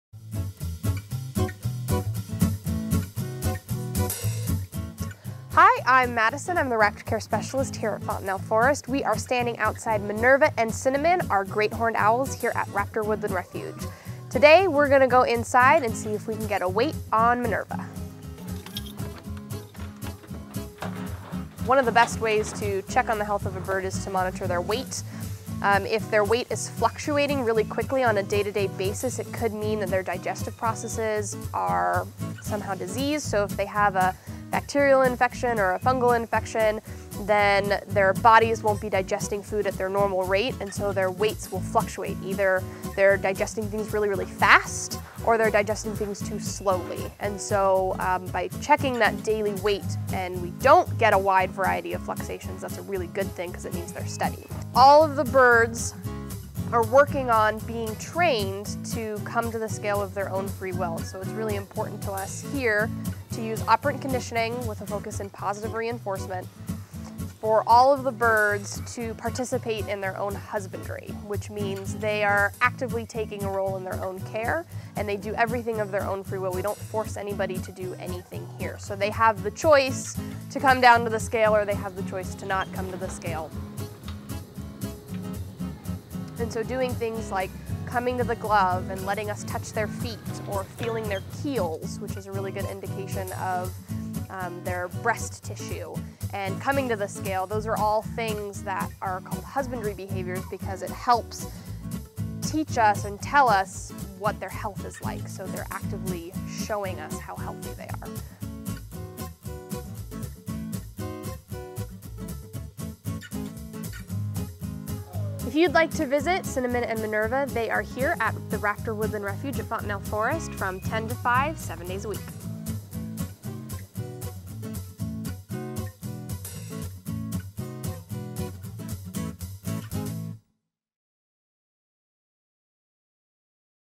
Meet the Raptors: Minerva and Cinnamon
In our first episode, we meet Cinnamon and her roommate Minerva. Sign up for our email list and stay current with all the latest happenings at the forest.